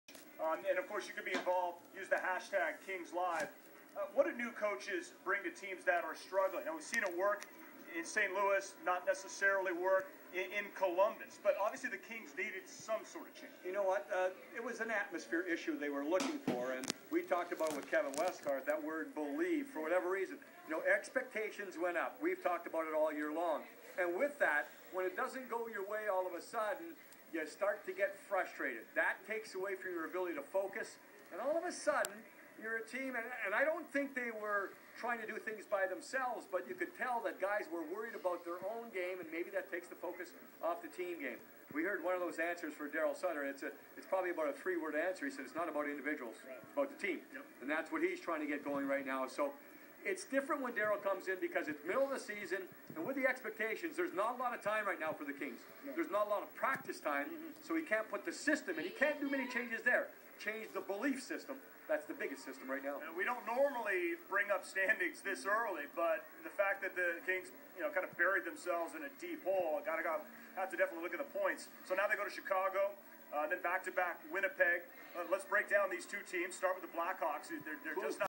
Discount Hockey On TV!!
gets featured on Live TV after an LA Kings game!